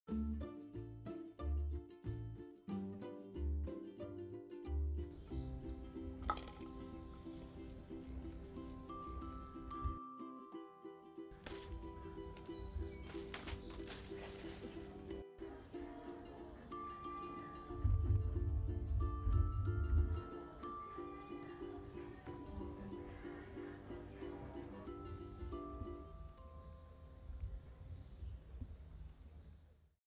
Lion Cubs Playing
5-month-old lion cubs Ken and Dixie received special enrichment in the form of gourds, paper bags with wood shavings, and unusual scents for them to search for and enjoy. The results were unbearably cute.